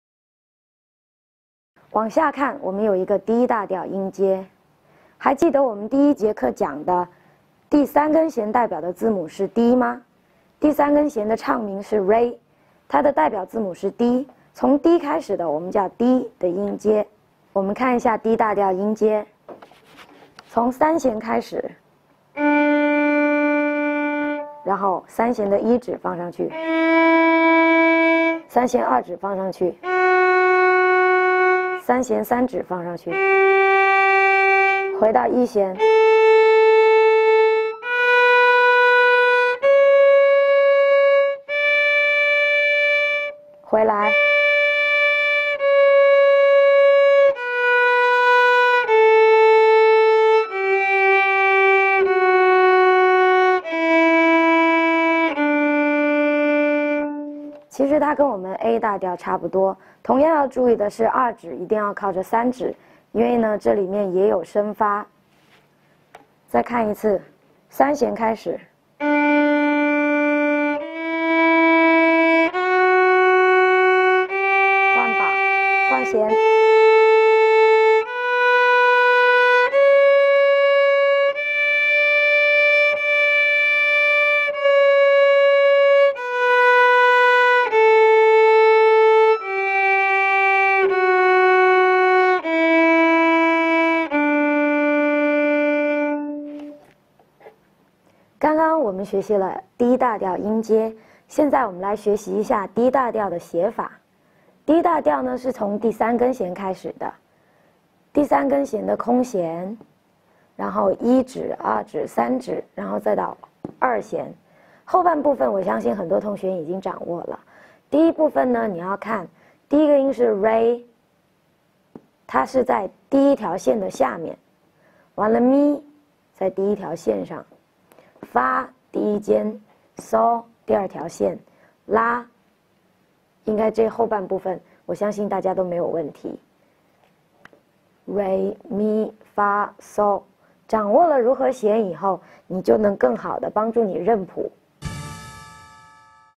铃木小提琴教材第1 2册37节
零基础学习小提琴视频教程，使用铃木（Suzuki VIolin）小提琴教材第1-2册